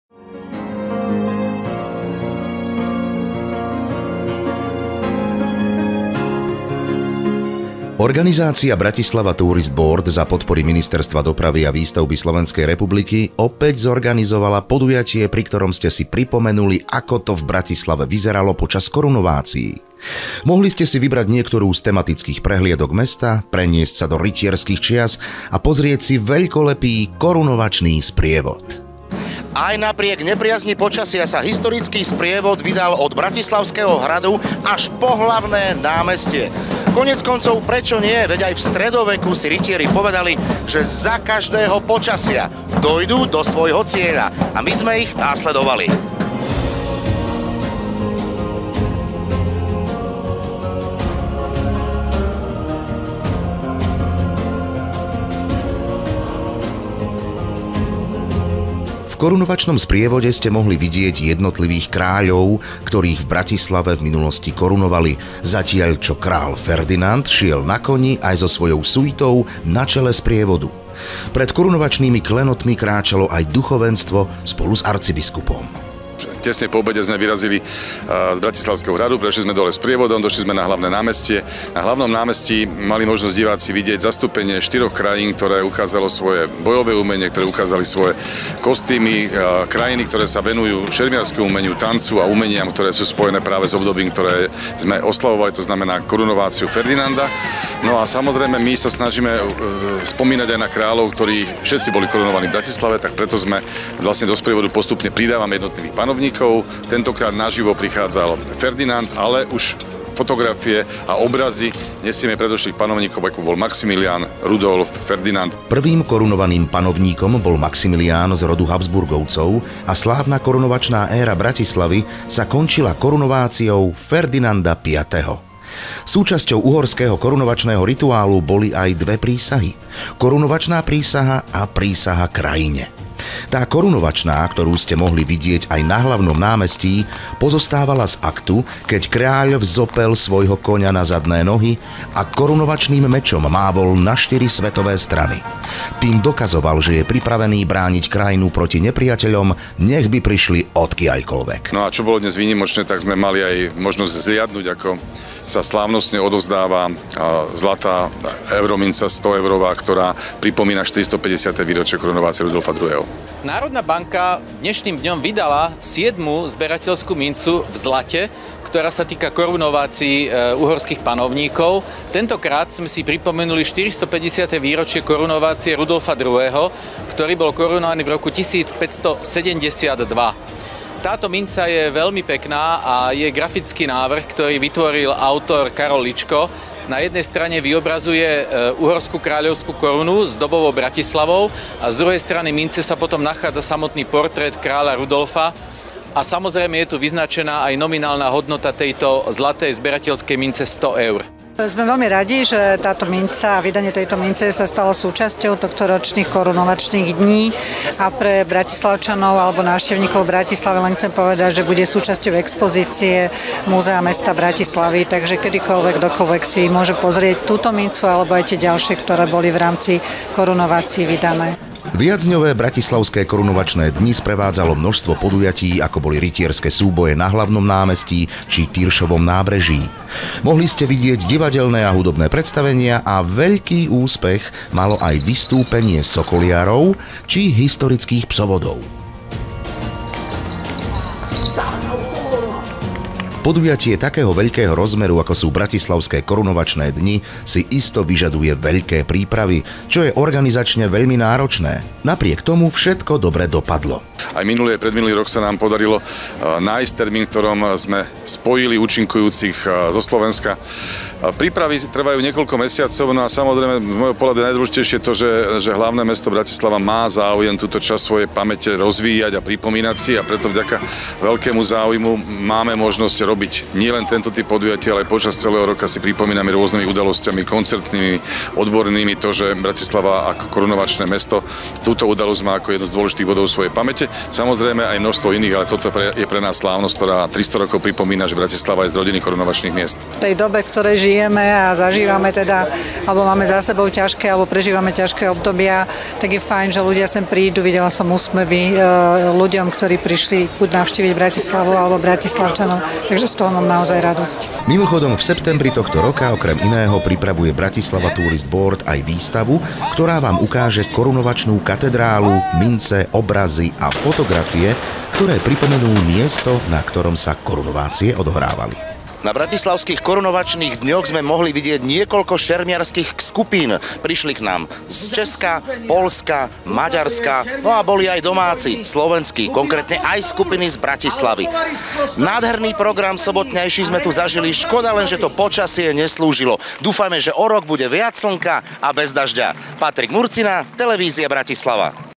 Bratislavské korunovačné dni 2022
Počas posledného júlového víkendu sa Bratislava opäť stala na chvíľu starým Prešporkom. V uliciach mesta ste sa vrátili o niekoľko storočí späť a to vďaka už tradičným Korunovačným dňom, ktoré sme sledovali spolu s našou kamerou.